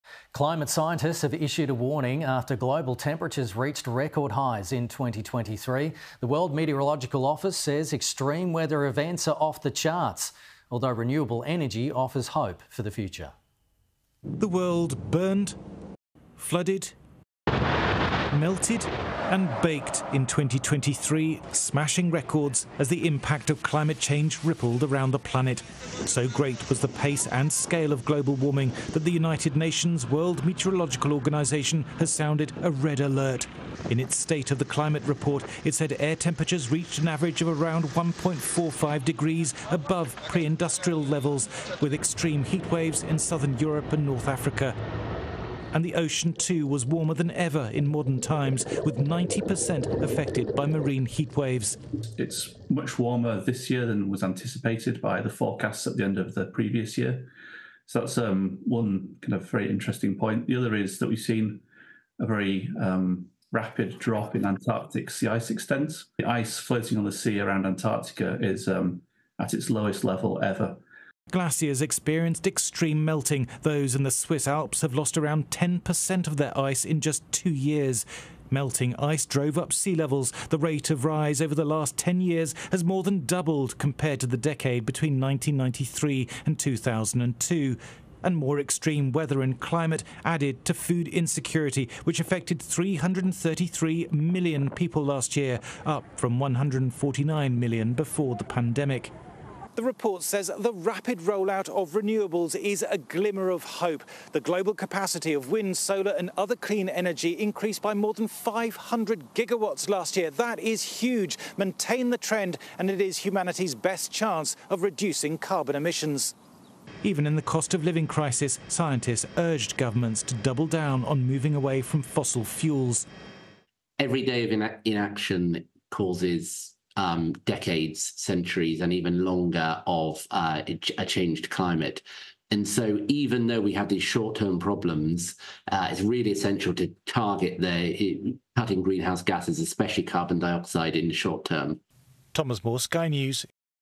Multiple weather records broken across the globe in 2023
Climate scientists have issued a warning after global temperatures reached record highs in 2023.

The World Meteorological Organization revealed extreme weather events are off the charts.

However, they said renewable energy offers hope for the future.

In its State of the Climate report, the WMO said air temperatures reached an average of 1.45 degrees above pre-industrial levels.

The ocean was also warmer than ever in modern times, with 90 per cent affected by marine heatwaves.